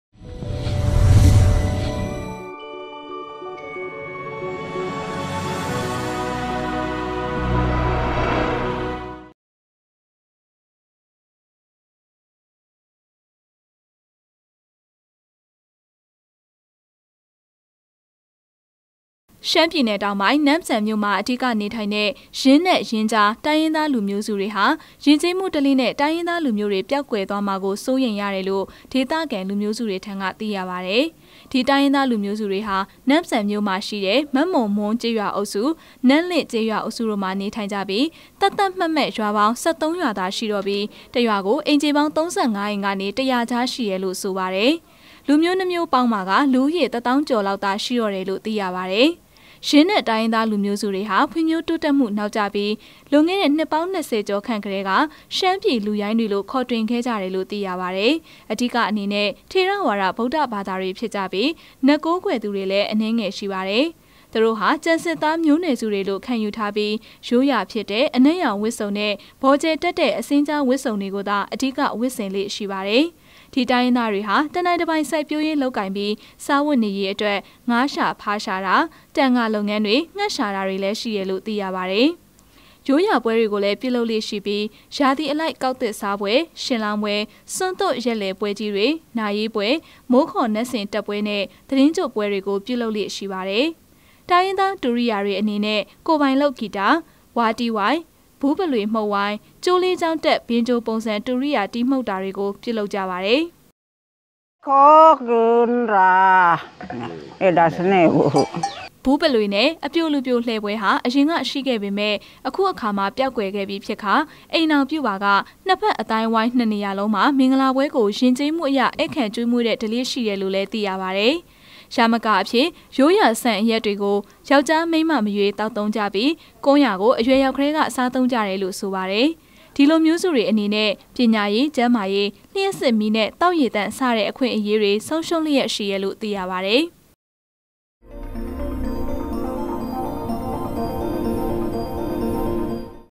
လူဦးေရ တစ္ေသာင္းေက်ာ္ေလာက္သာ က်န္ရွိေတာ့သည့္ ...
လူဦးေရ တစ္ေသာင္းေက်ာ္ေလာက္သာ က်န္ရွိေတာ့သည့္ ယင္းနက္၊ ယင္းၾကား တုိင္းရင္းသား လူမ်ဳိးစုမ်ား
ရွမ္းျပည္နယ္ေတာင္ပုိင္း၊ နမ့္စန္ၿမိဳ႕မွာ အဓိက ေနထုိင္တဲ့ ယင္းနက္၊ ယင္းၾကား တုိင္းရင္းသား လူမ်ိဳးစု ေတြဟာ ယဥ္ေက်းမႈဓေလ့နဲ႔ တိုင္းရင္းသား လူမ်ဳိးေတြ ေပ်ာက္ကြယ္သြားမွာကို စိုးရိမ္ရတယ္လုိ႔ ေဒသခံ လူမ်ိဳး စုေတြထံက သိရပါတယ္။
ဒီတုိင္းရင္းသားလူမ်ိဳးစုေတြဟာ နမ့္စန္ၿမိဳ႕မွာရွိတဲ့ မက္မုန္မြန္း ေက်းရြာအုပ္စု၊ နမ့္လစ္ေက်းရြာ အုပ္စု တုိ႔ မွာ ေန ထုိင္ၾကၿပီး သတ္သတ္မွတ္မွတ္ ရြာေပါင္း ၁၃ ရြာသာ ရွိေတာ့ၿပီး တစ္ရြာကို အိမ္ေျခေပါင္း ၃၅ အိမ္ ကေန ၁၀၀ ၾကား ရွိတယ္လုိ႔ဆုိပါတယ္။ လူမ်ိဳးႏွစ္မ်ဳိးေပါင္းပါက လူဦးေရ တစ္ေသာင္းေက်ာ္ေလာက္သာ ရွိ ေတာ့တယ္လုိ႔ သိရပါတယ္။